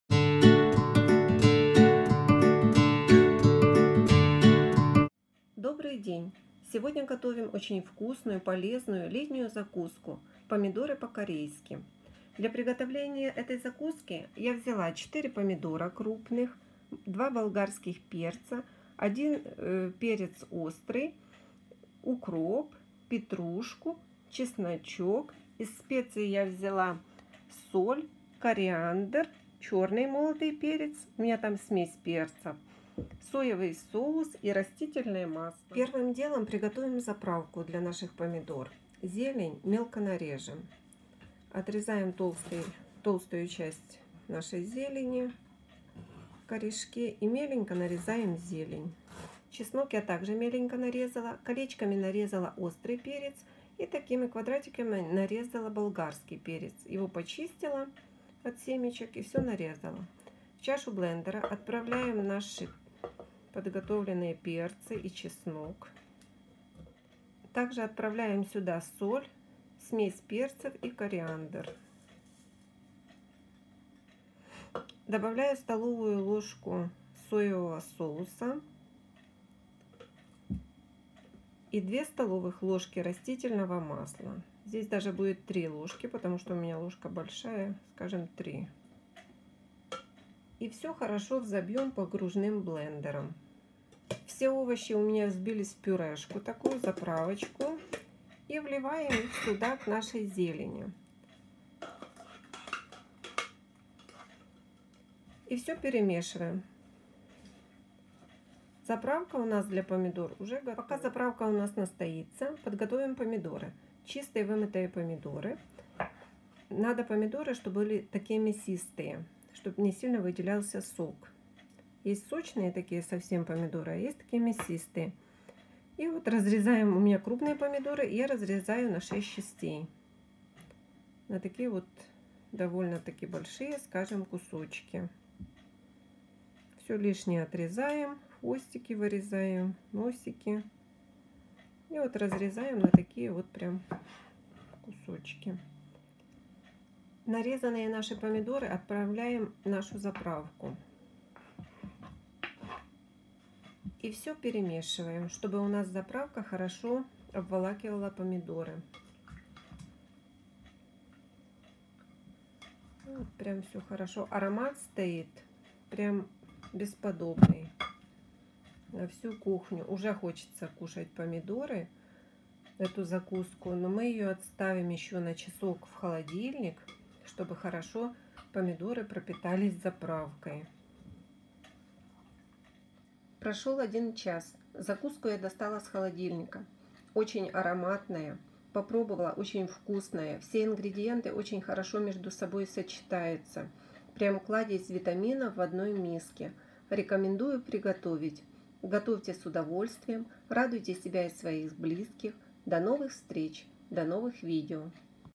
Вкуснейшая закуска!Помидоры по-корейски! Простой рецепт вкусной закуски. Рецепты. Готовим с Инной
Ингредиенты :
1 кг помидор
200 г болгарского перца (. 2 шт)
30 г петрушки
30 г укропа
1—2 зубка чеснока
1/2 ч л соли
1/2 ч л кориандра
1/3 ч л перца
Острый перец
2—3 ст л растительного масла
1 ст л соевого соуса
Приятного аппетита!